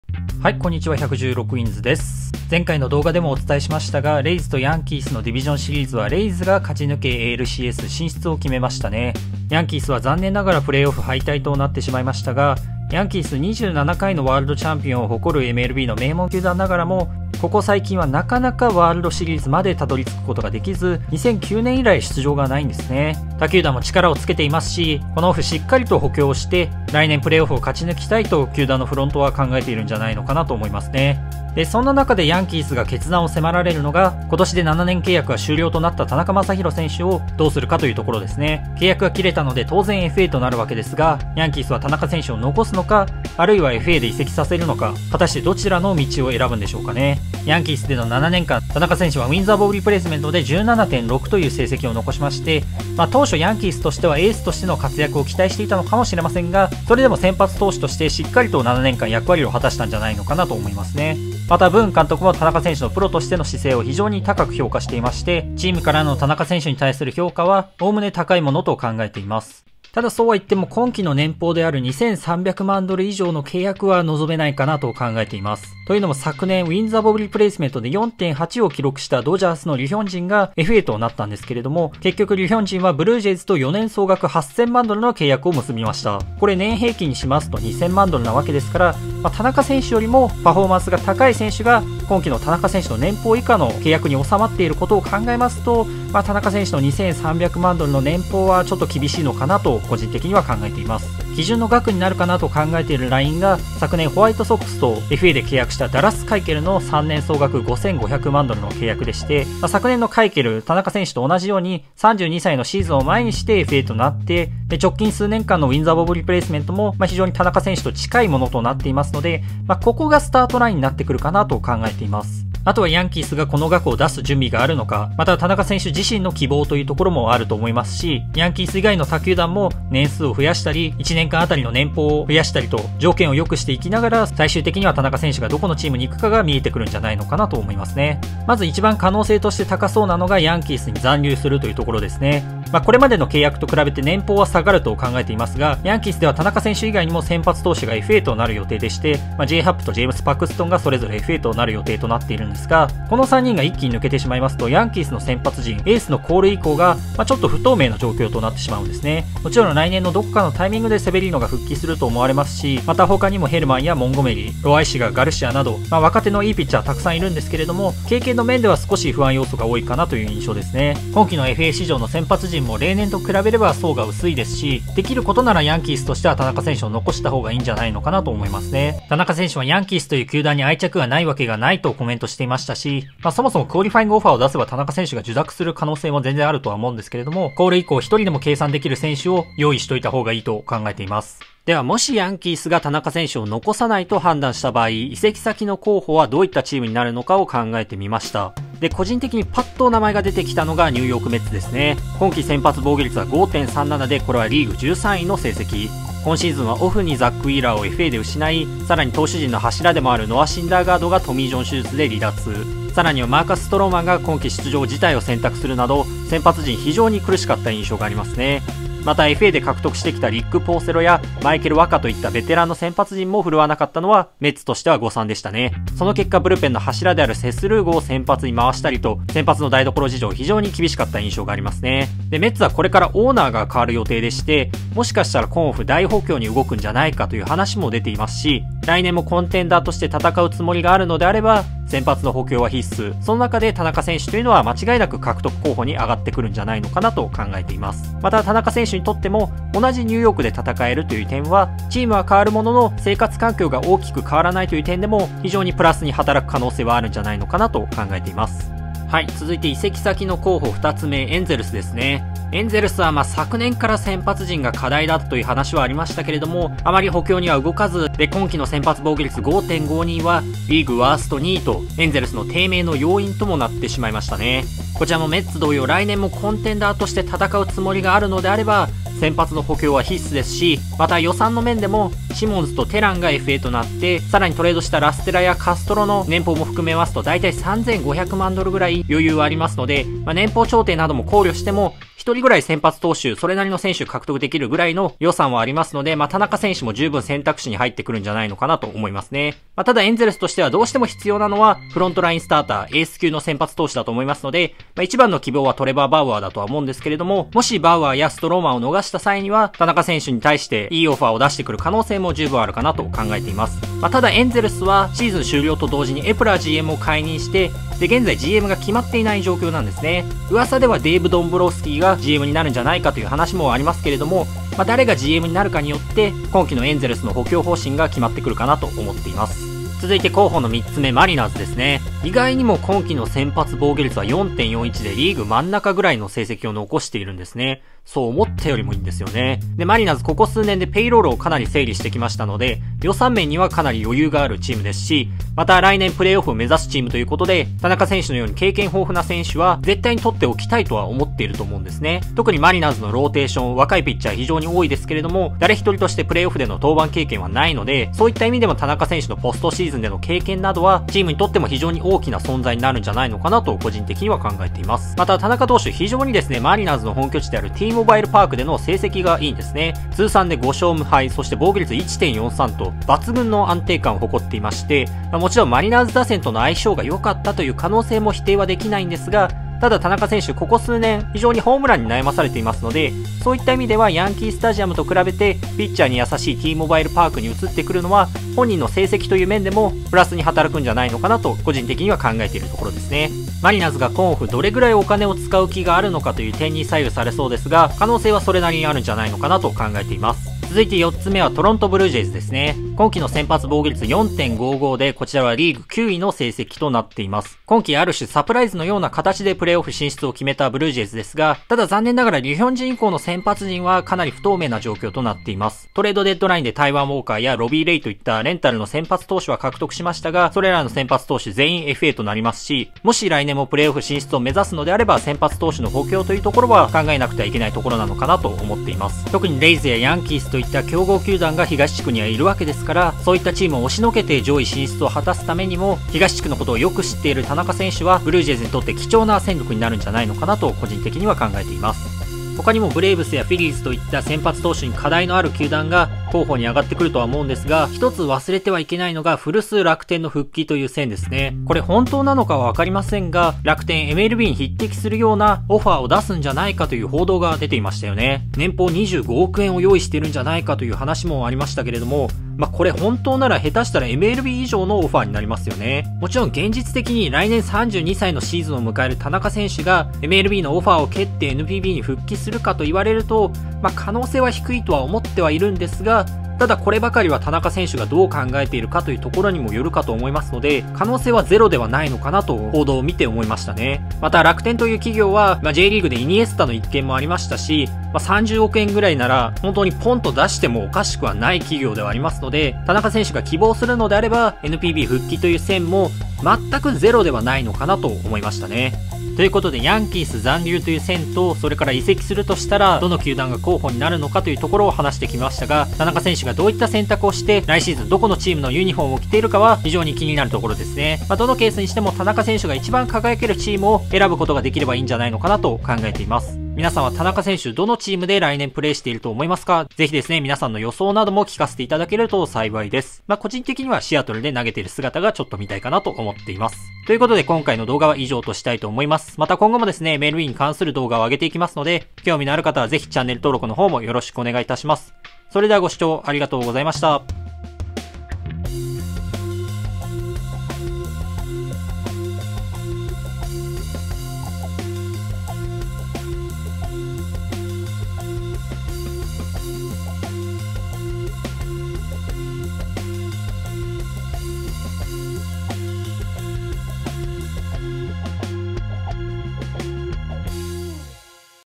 【MLB】田中将大選手はFAでどこへ行く？
日曜か月曜に上げようと思って準備していましたが、色々あって遅くなってしまいました。

ヤンキースは今年もプレーオフで敗退となり、2009年以来ワールドシリーズから遠ざかっている状況です。今オフはそんなチームから7年間先発陣を支えてきた田中将大選手もFAとなる予定で、残留か移籍かが注目されます。

残留の可能性が一番高いと思っていますが、移籍先の候補も考えてみました。